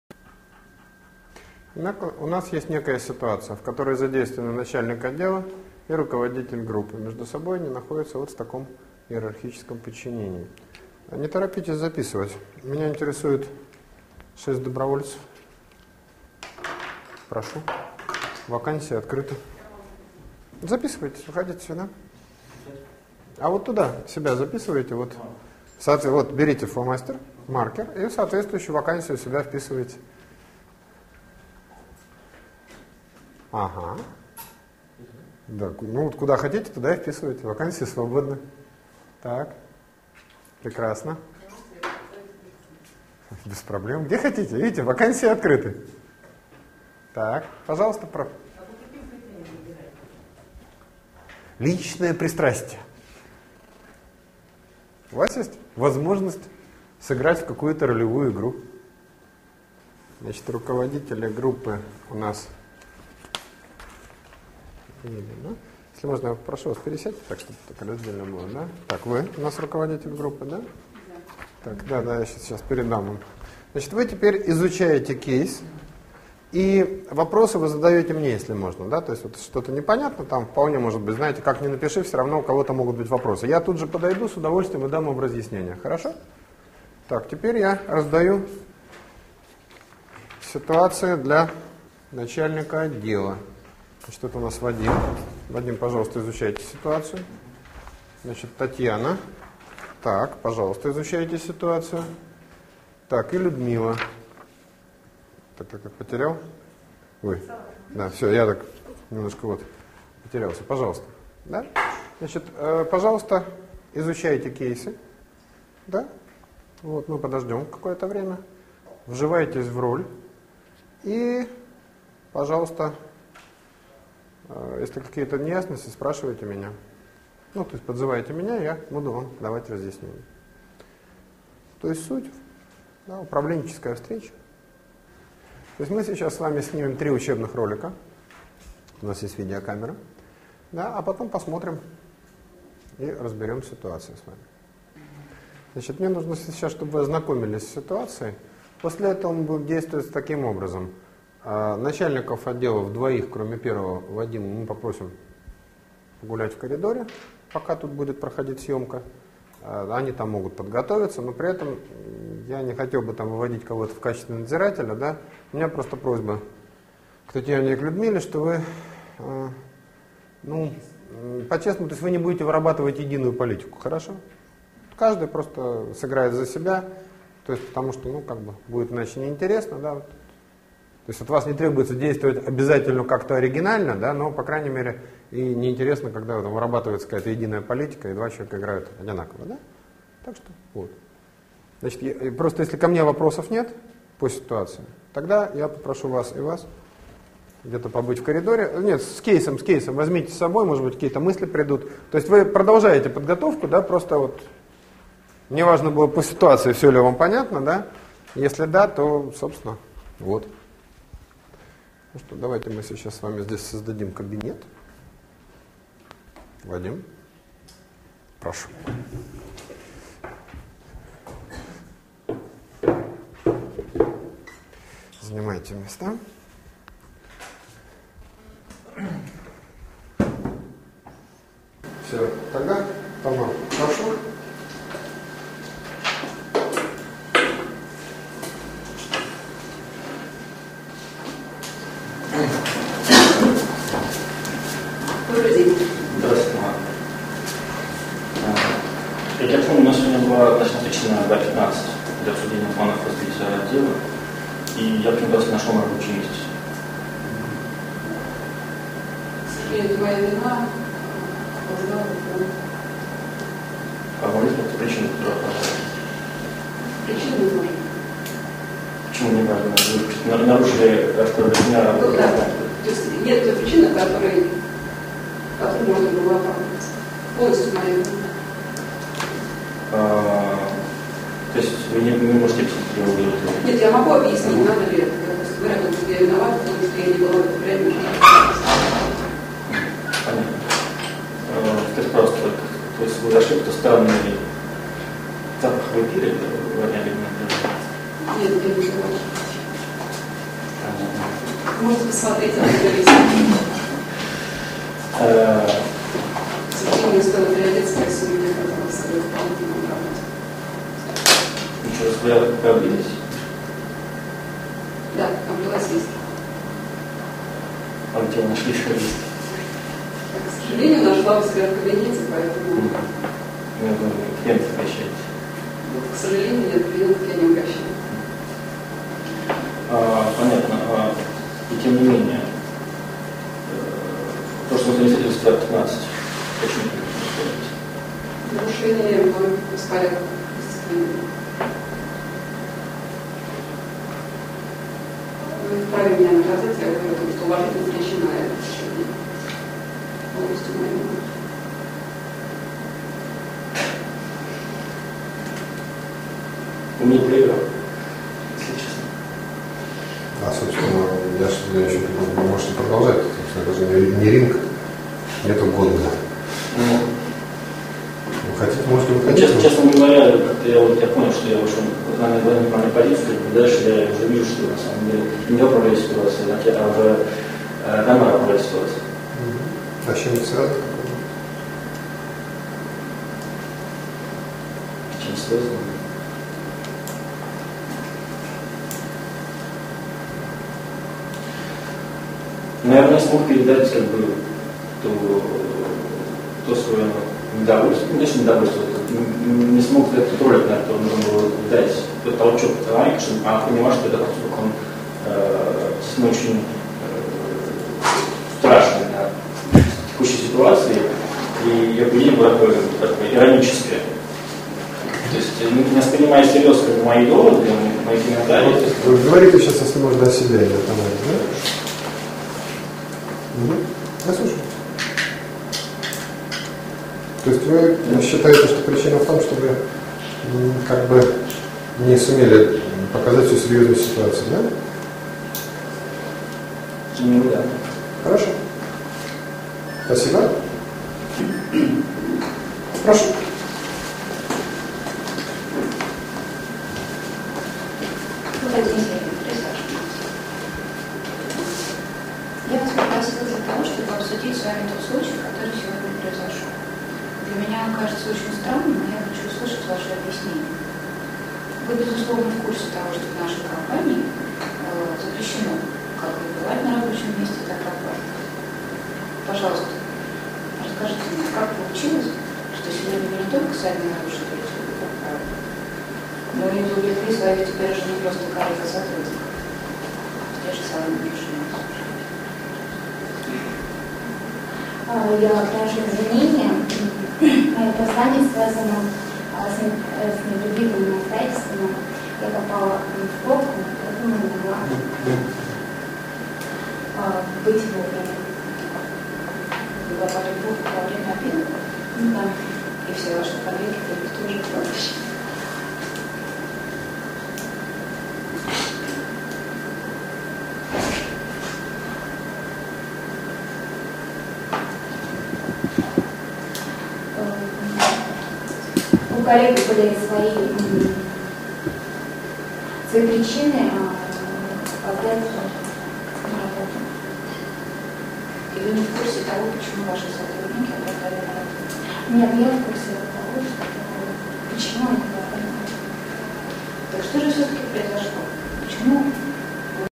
Effective HRM (Human Resource Management) RUS Part 22
А. Фридман. «Эффективное УПРАВЛЕНИЕ ПЕРСОНАЛОМ: интенсив для руководителей!»
В данном видеосеминаре впервые рассматривается полный комплекс возможных проблем руководителя, связанных с управлением персоналом, и предлагаются сверхэффективные методики их решения. Темы объединены в единую концепцию, что позволяет эффективно и интегрированно использовать весь спектр существующих инструментов управления.
Цель обучения - максимально увеличить отдачу от действий персонала за счёт повышения эффективности управления и развития управленческой квалификации руководителей всех уровней!